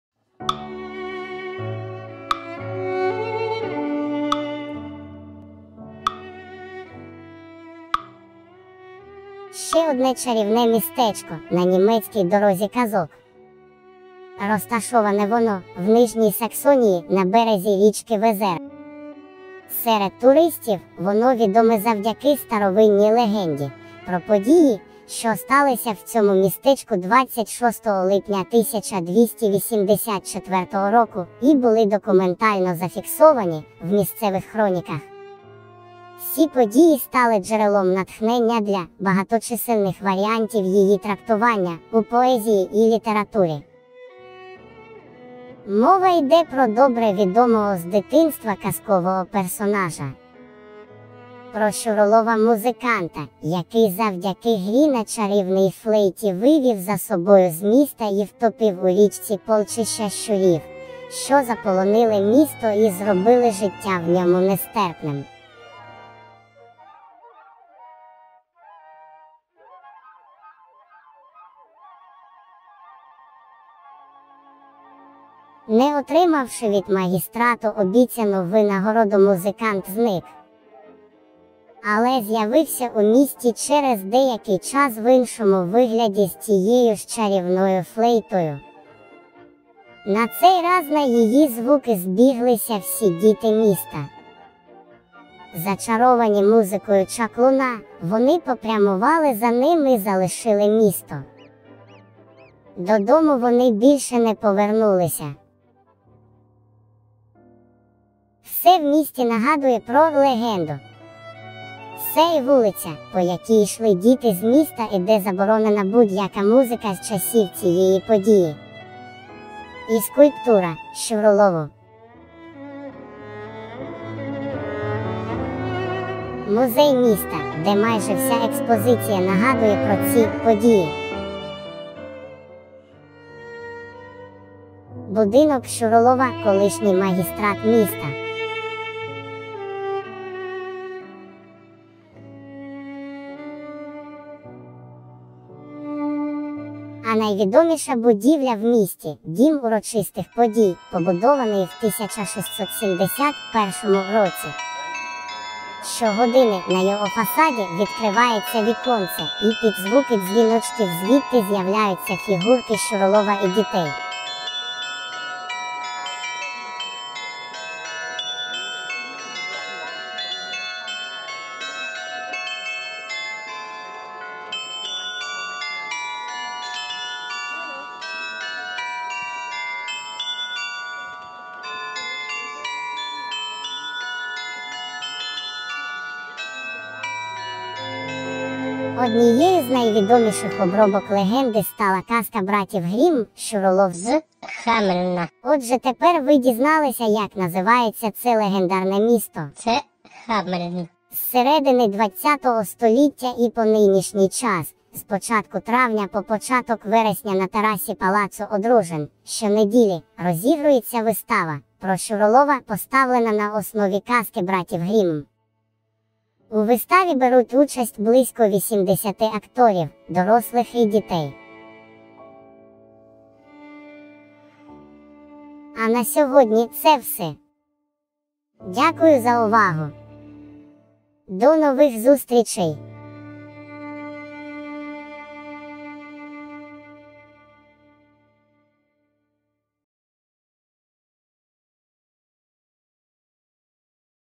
Подорожі з Гноміком. Місто...?. Дорога Казок. Німеччина. Земля Нижня Саксонія.
Ще одне мальовниче містечко на Дорозі Казок.
Німецька дорога казок проходить через міста, селища, старовинні фортеці і замки, в яких в стародавні часи жили герої казок братів Грімм та інші казкові персонажі. Повсюдно казковим героям встановлюють пам'ятники з бронзи або каменю, вони стають візитною карткою міста і місцем паломництва туристів.
Туры в Европу, Туры в Чехию, Туры в Прагу и Карловы Вары